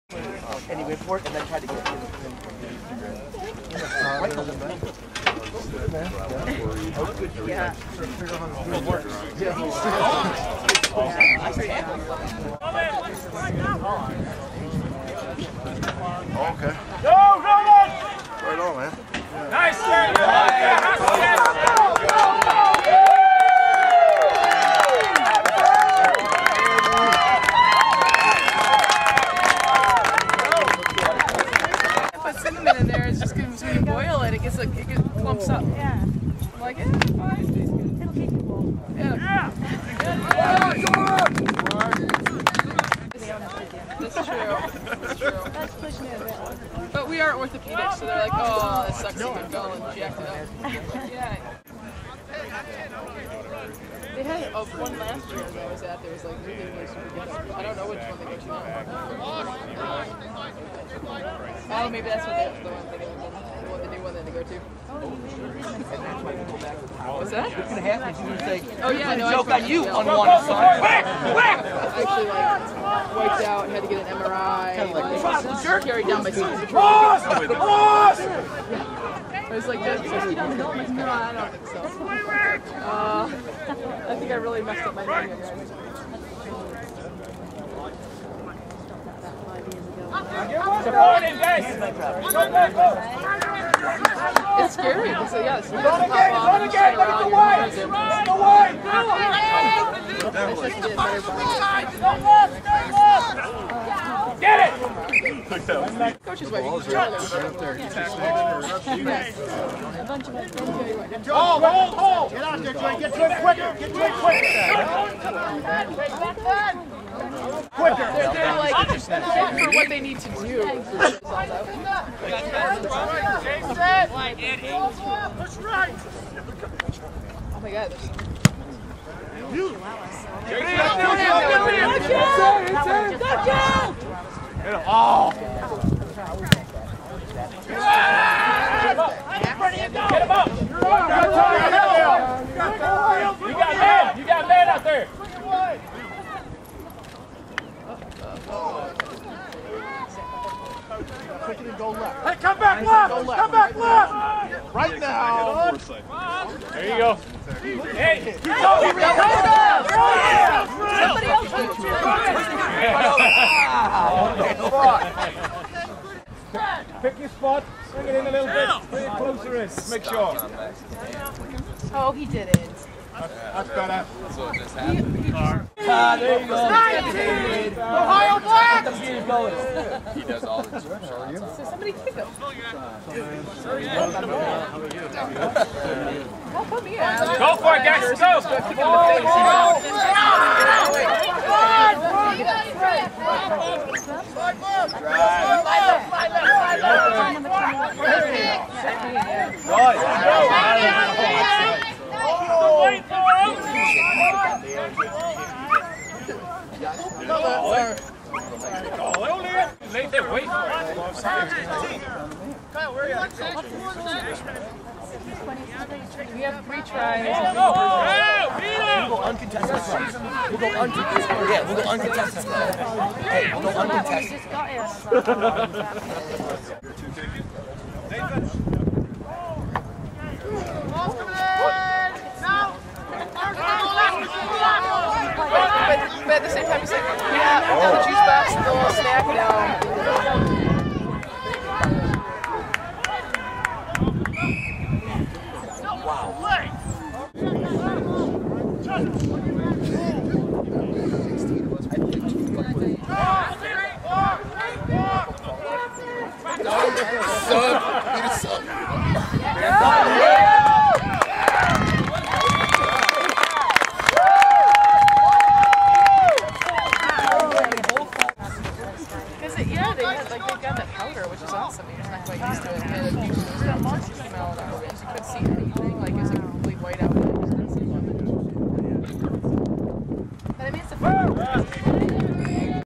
DRFC vs Flint 02 April 11 pt 4 of 8
Matthew-Ryan Mathis scores his ZULU TRY! and Liam scores as well!